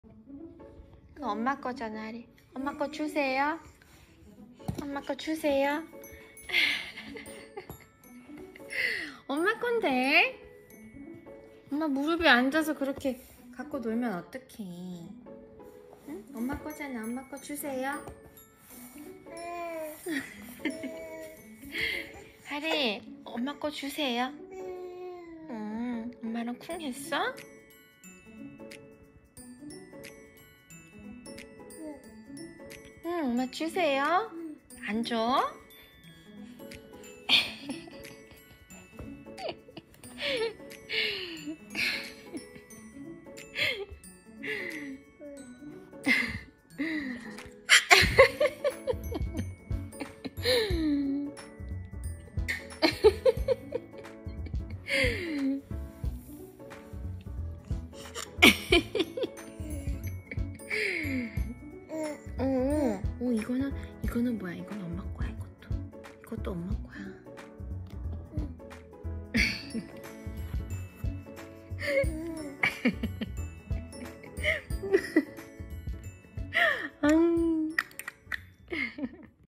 그토록 만져보고 싶었던 엄마의 네모 장난감
D+419

엄마가 매일 꽁꽁 숨기고 안 보여줬던 네모난 장난감
저게 도대체 뭘까 뭔데 엄마아빠가 매일 보고 있을까

궁금해서 만져보고 싶었는데
엄마는 도무지 보여줄 생각을 안 하네!

엄마가 잠시 자리 비운 사이에 냉큼 잡고는
엄마가 달라 해도 주지 않는 하리

휴대폰 잡고 까악 하고 웃어도 보고
엄마한테 메롱도 해보고
야호 나도 얇은 네모난 장난감 만져봤다🤓 하는듯👶🏻💓

bgm 출처 : 오늘의 일기 @oneul274